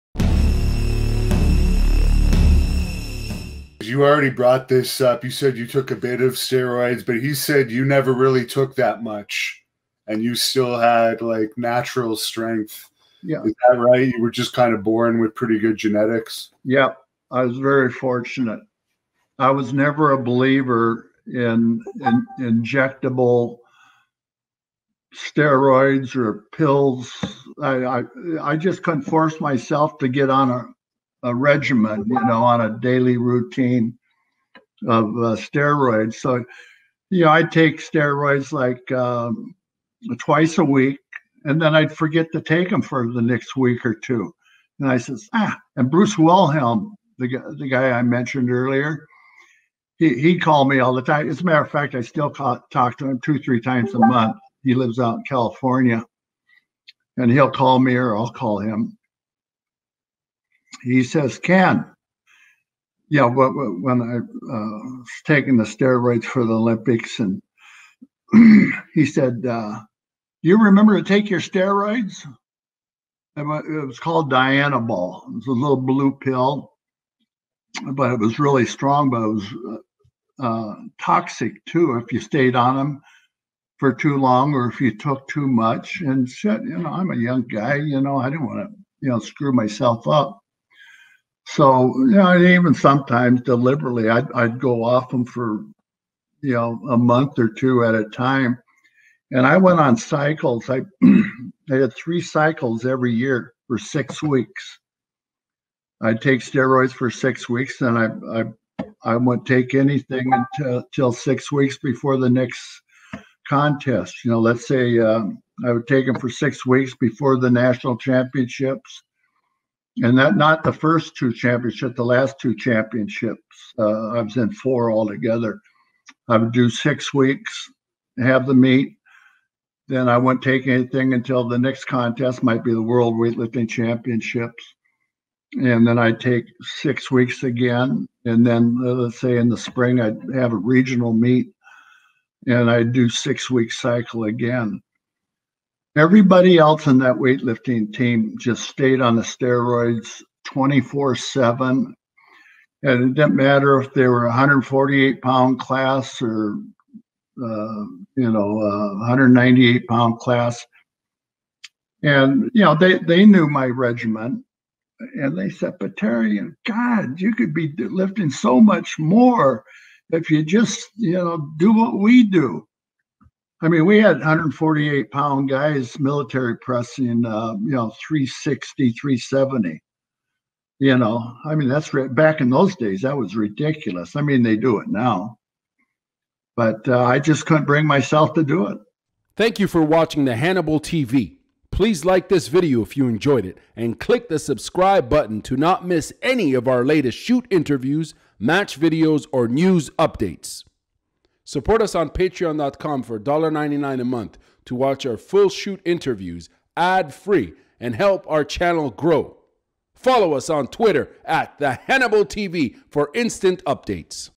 Ken Patera Discusses Steroids'
Former Olympian and Legendary Wrestler Ken Patera discusses weight lifters steroid use as well as his own cycle philosophy over the years.